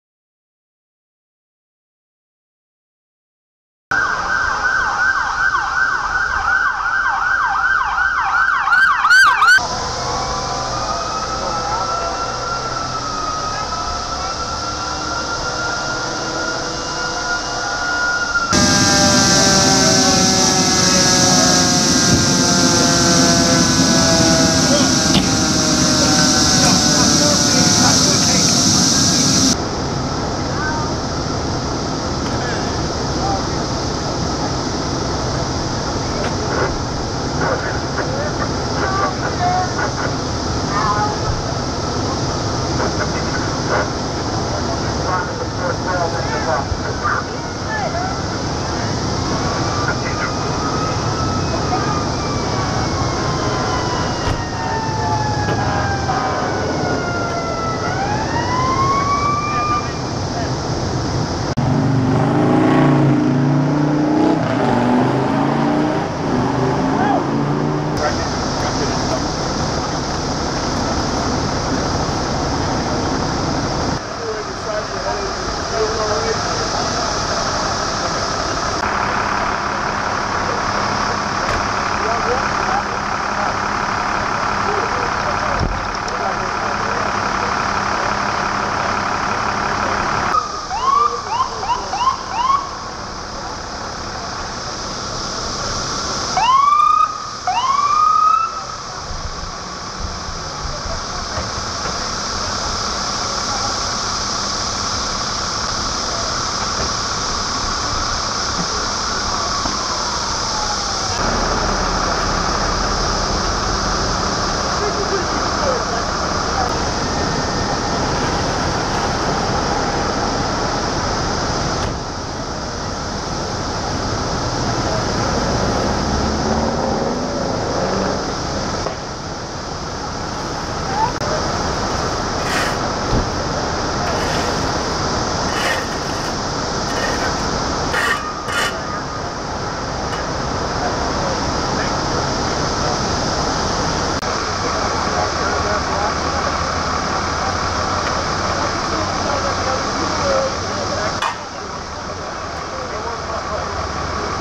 Pre Arrival AT 2 CAR COLLISION WITH CAR OVERTURNED
What A two vehicle collision  leave one vehicle overturned  and multiple people injuries at least three people including a child call transported to the hospital  unknown severity of injuries

Where Springfield Street and RT 309  / time 3 pm

Twitter LLN PHILLY